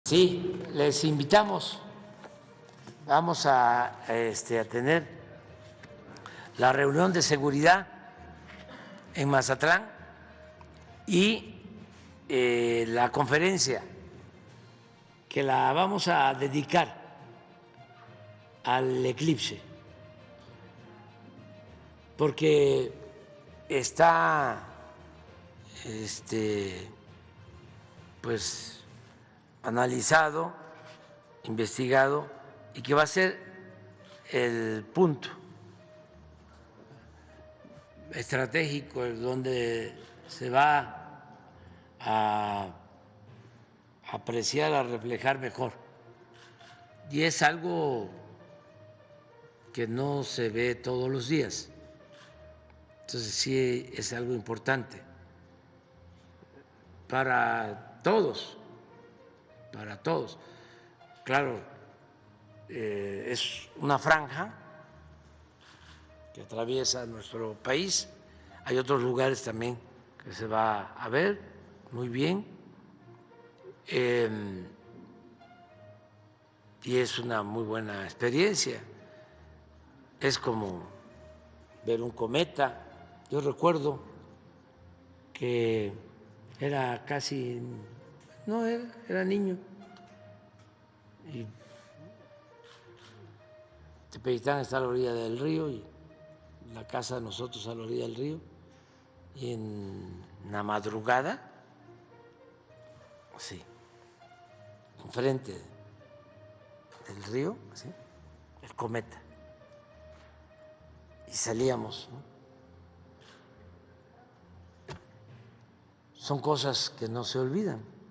"Vamos a tener el elipse en transmisión nacional"; AMLO estará el Mazatlán el 8 de abril
El presidente Andrés Manuel López Obrador confirmó este lunes que el
próximo 8 de abril estará en Mazatlán, Sinaloa, para apreciar el eclipse solar
total, a invitación del gobernador Rubén Rocha Moya.
López Obrador refirió que ese lunes 8 de abril la conferencia de prensa
mañanera se realizará en el puerto de Mazatlán y luego se podrá apreciar el
fenómeno.